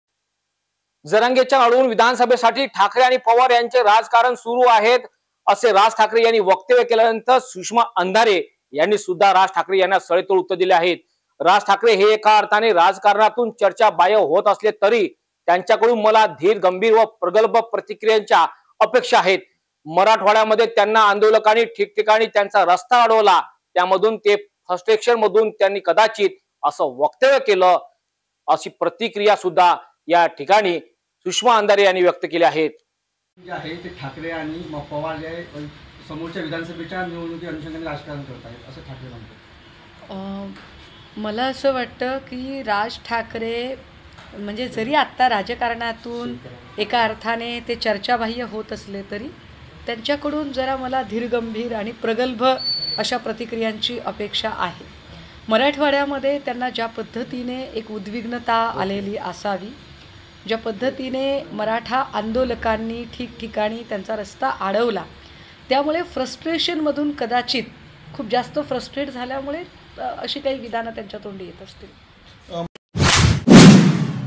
राज_ठाकरे राजकारणातून चर्चा बाह्य व्यक्ती सुषमा_अंधारे यांचे ठाकरेंना उत्तर
जरांगे असून विधानसभे साठी ठाकरे पवार यांचं राजकारण या टीकेवर राज ठाकरेंना दिले उत्तर
मराठवाड्यात राज ठाकरेंना लोकांनी अडविले त्याच फ्रास्टेशन वक्तव्य केलं असेल
Edit
Anchor
Upload